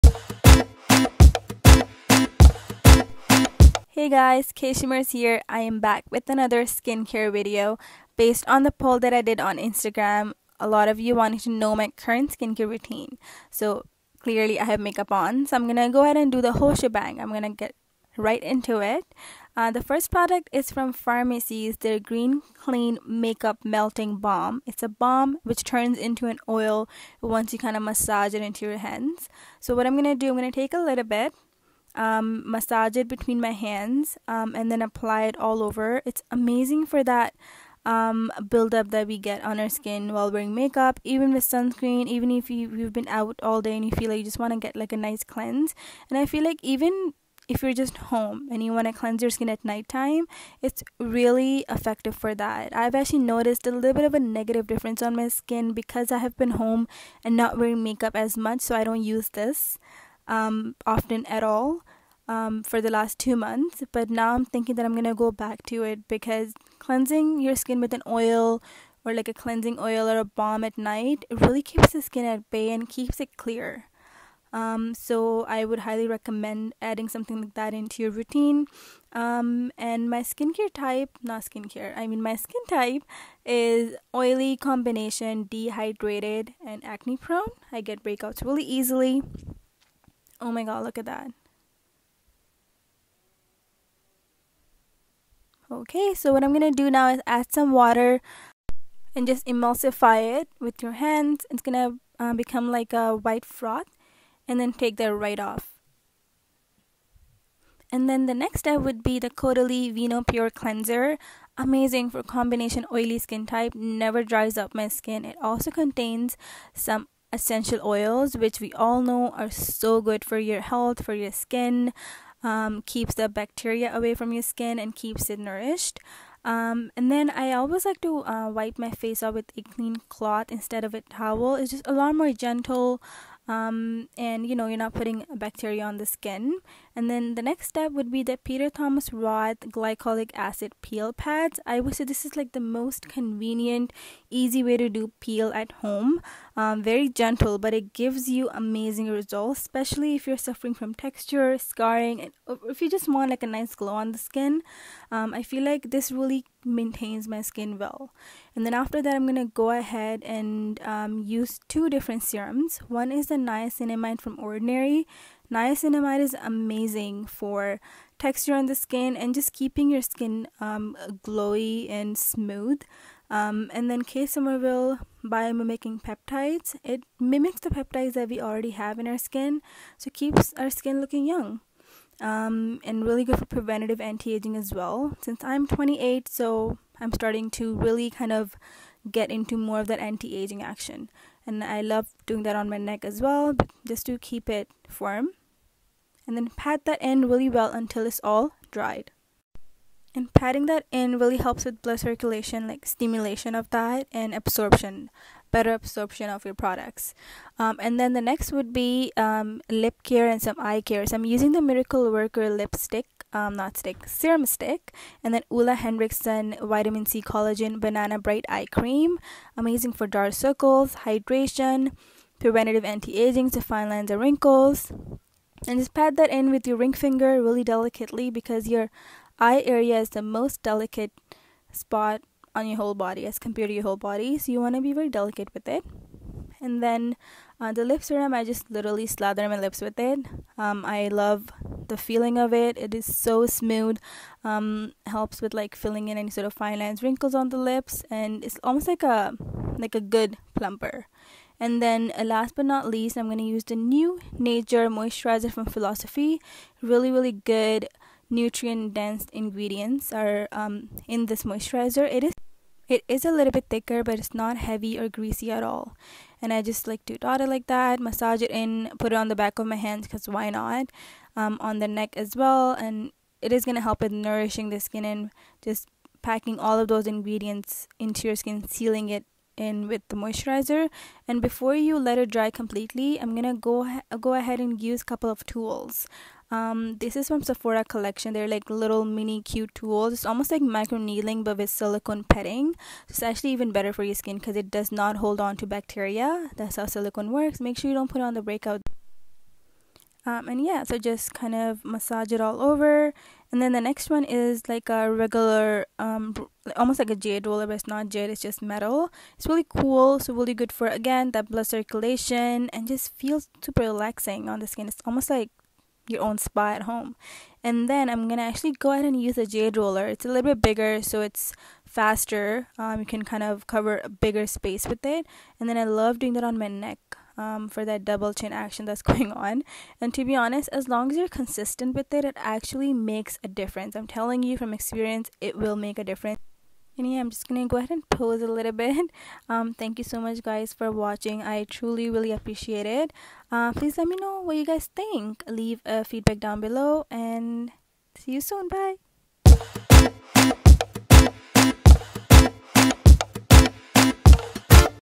Skincare for scarring and acne - let’s build a routine
Hey guys, I have been using these products and tools for a while now and they have been doing wonders for my skin so many of you wanted to know my whole routine so here it is!!!